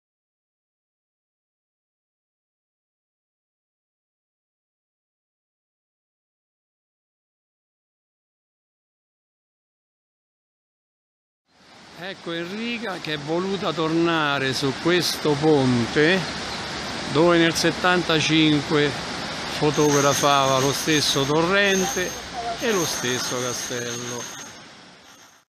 VLOG-saluti dall'Alto Adige 15 agosto 2018
su uno dei ponti di Campo Tures dove nel lontano 1975 scattai la prima foto...